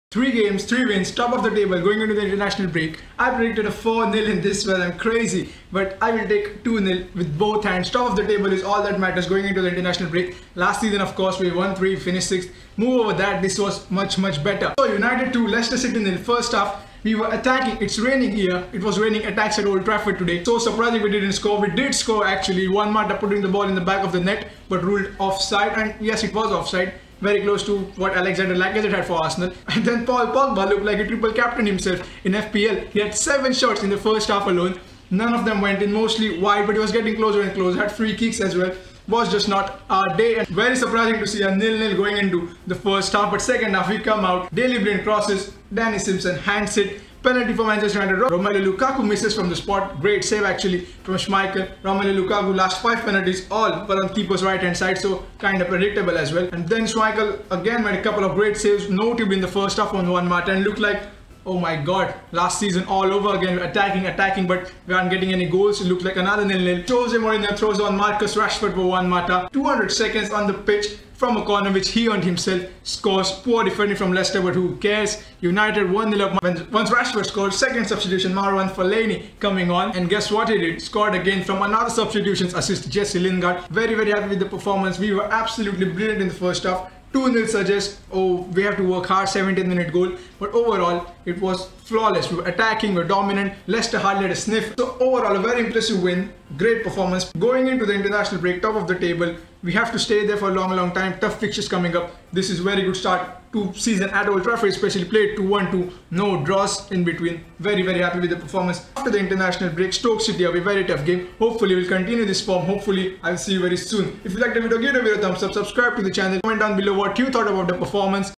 Manchester United 2 : Leicester City 0 Review : Super Subs Win the Game for United !!
Man Utd 2 : Leicester City 0 , TOP OF THE TABLE ! 3 WINS OUTTA 3 ! COME ONNN UNITED !

To Connect personally ,